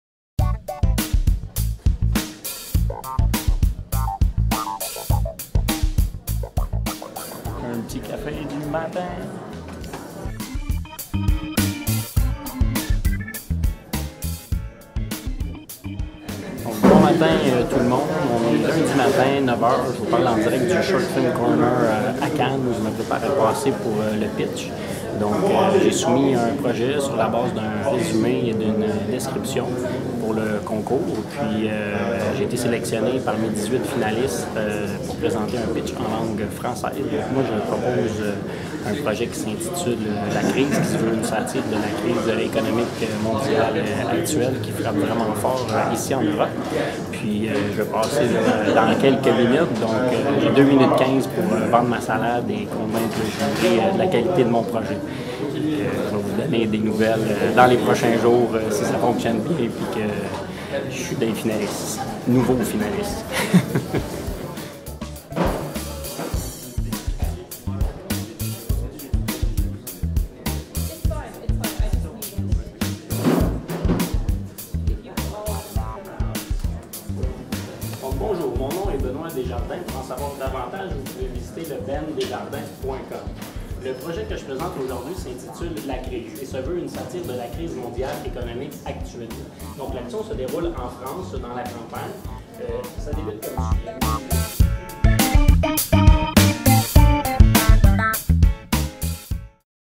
Le PITCH - Cannes Short Film Corner
Ma participation au concours le Pitch en direct du Short Film Corner à Cannes où je présente le projet: La Crise.